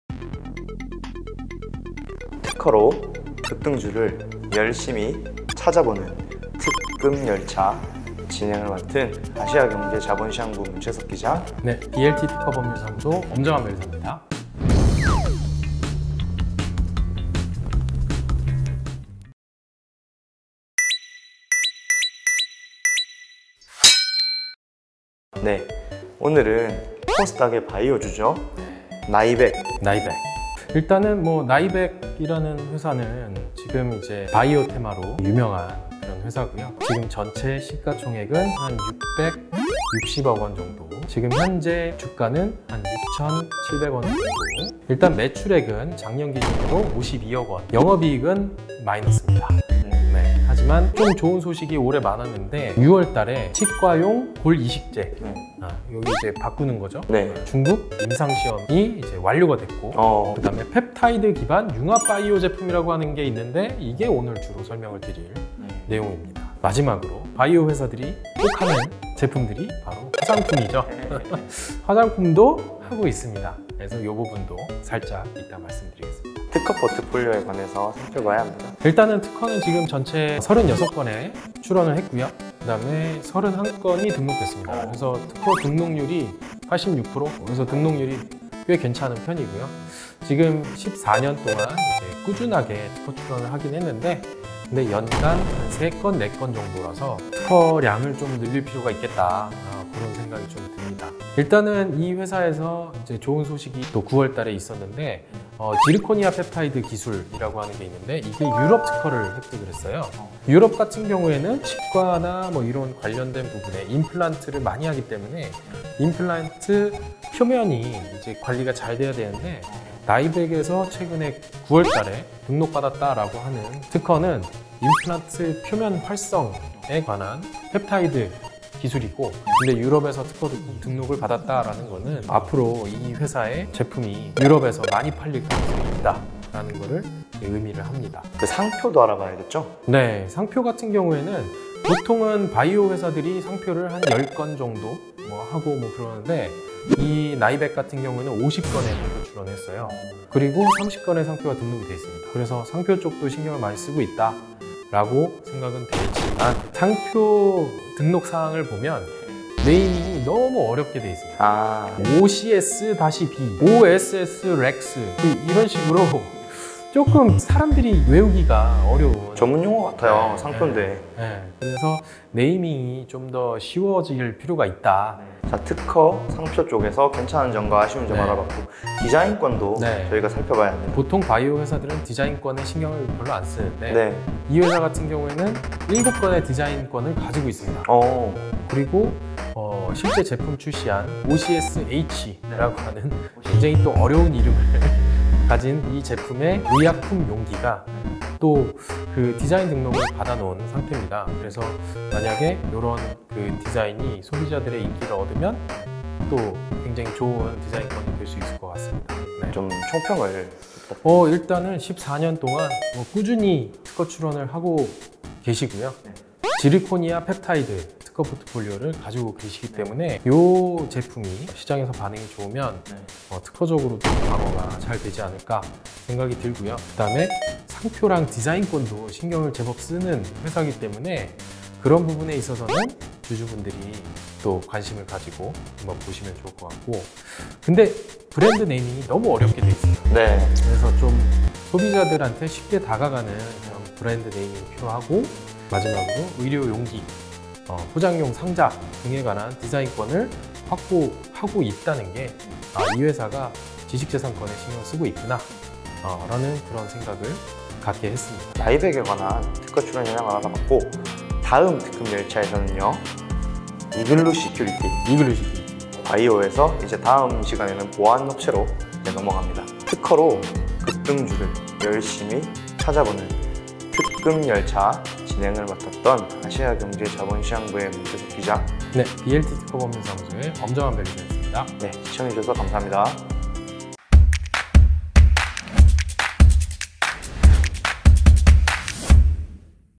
[특급열차] ㉘ 나이벡 - 고령화 시대의 수혜 분석
특허로 급등주 열심히 찾아봅시다!
BLT특허법률사무소 엄정한 변리사와,
아시아경제 문채석 기자가 특허를 통해 상장사를 분석합니다.

‘특급열차’의 스물여덟 번째 정류장은 ‘나이벡’입니다.
튼튼한 바이오 기업이죠.

나이벡은 2019년 치과용 골 이식재를 개발해 중국 임상시험까지 마쳤는데요.

펩타이드를 기반으로 한 융합 바이오 사업에서 두각을 보이고 있습니다.

꾸준한 특허 출원과 관리를 해온 이 기업.
변리사님이 나이벡에 추천하는 사업 전략은 무엇일까요?

꿀케미, 꿀재미, 꿀정보 모~두 보장합니다!
주식시장이 궁금한 주주님들, ‘특급열차’에 주목해 주세요!